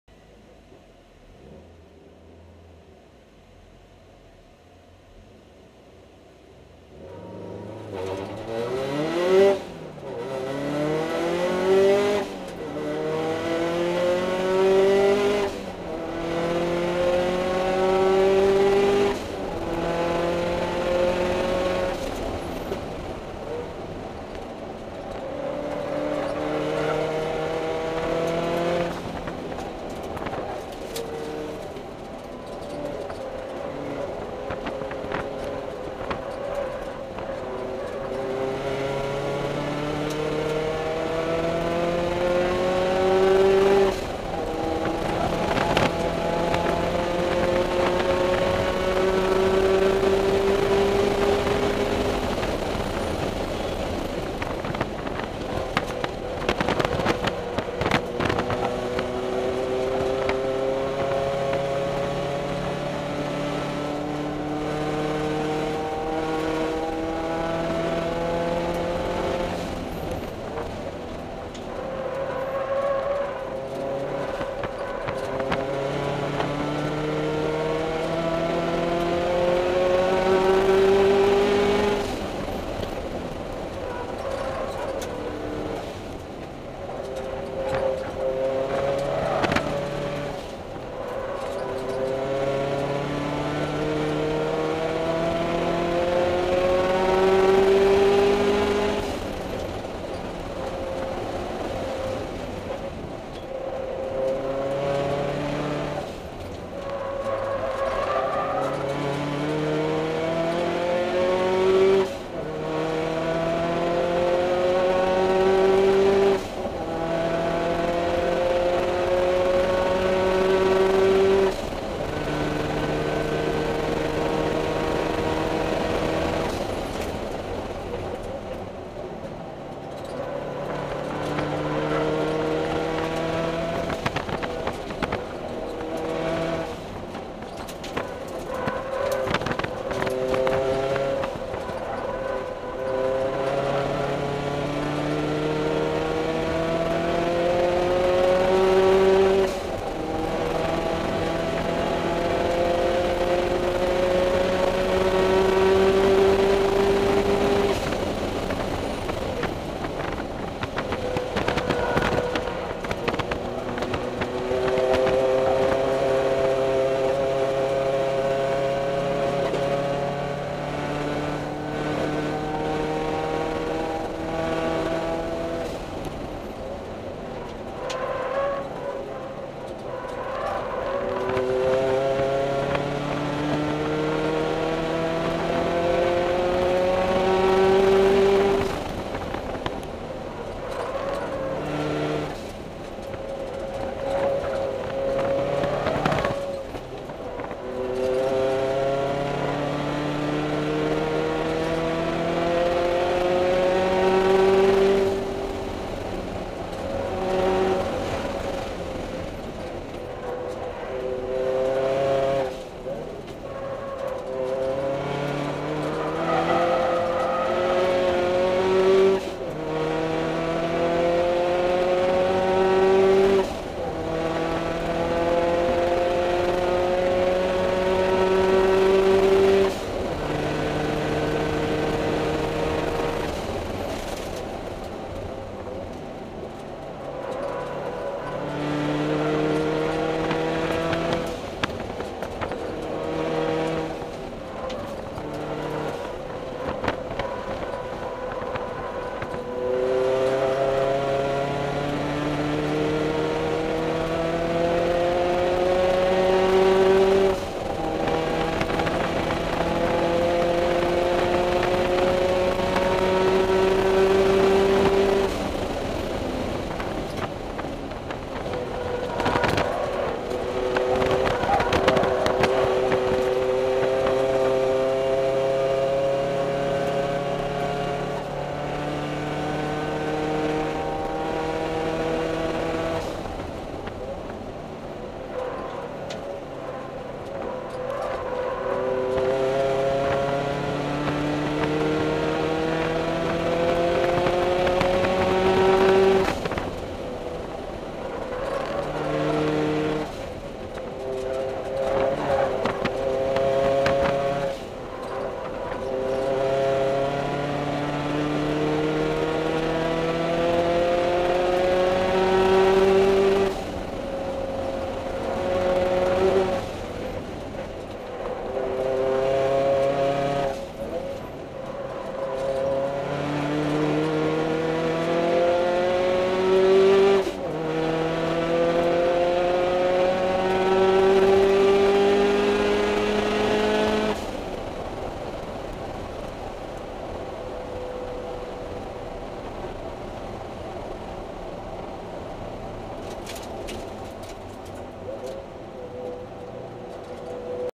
Toyota Production Engineers race in One Lap of America- Mid-America Motorplex
Toyota Production Engineers are running the TRD Scion FRS in the One Lap of America this week. Sunday, May 3rd, the team took on a 2.23 mile circuit at Mid-America Motorplex, just outside of Omaha, Neb., with official times of 5:32.863 in the morning and 5:24.061 in the afternoon. That puts Toyota PE in first place in the SSGT2 Small Bore class and 9th overall after day two. One Lap of America covers nearly 3,500 miles and nine race tracks.